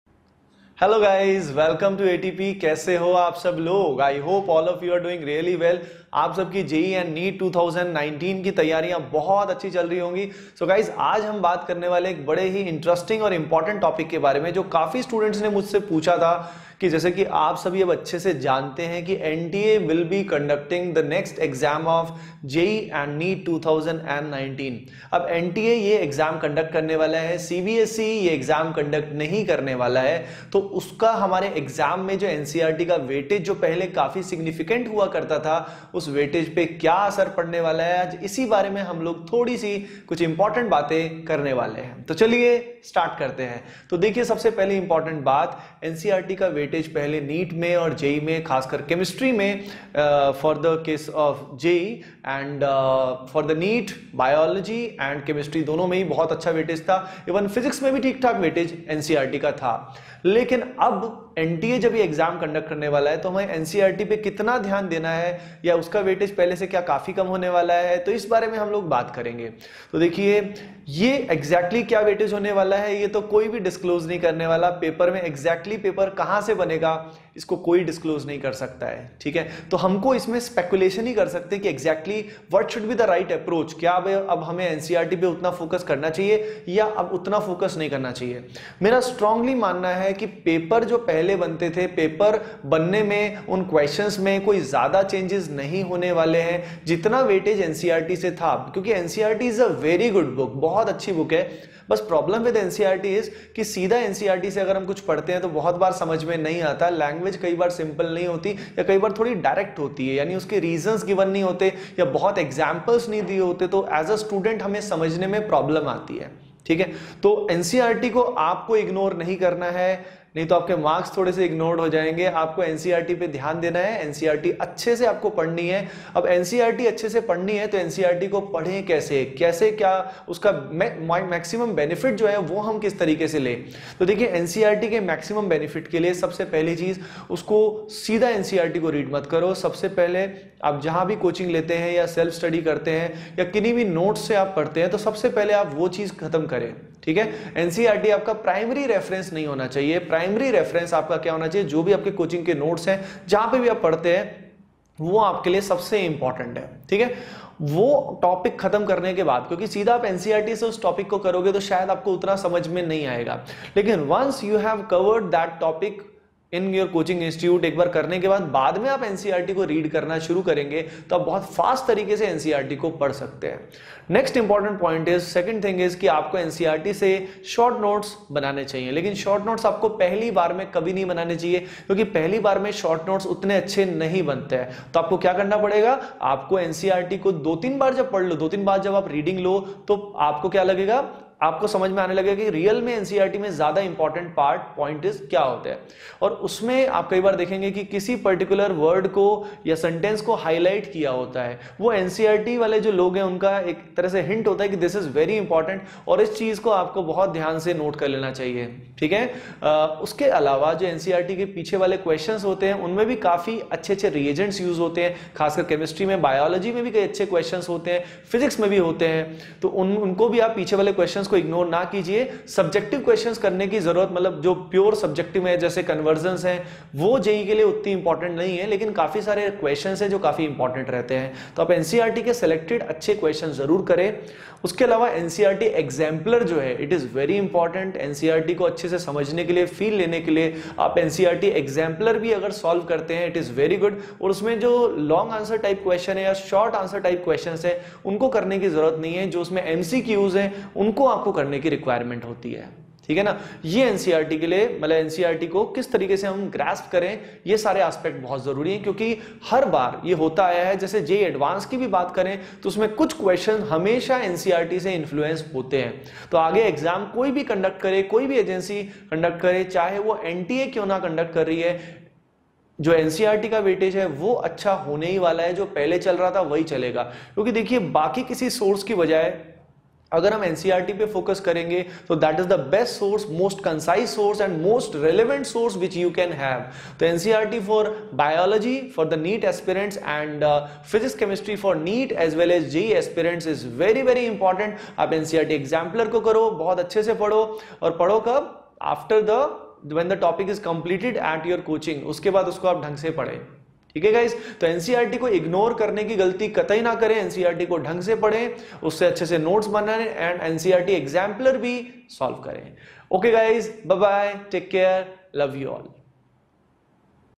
After NTA - Weightage of NCERT Reduced ? IIT JEE | Vineet Khatri Sir | ATP STAR Kota
atpstarjee jee mains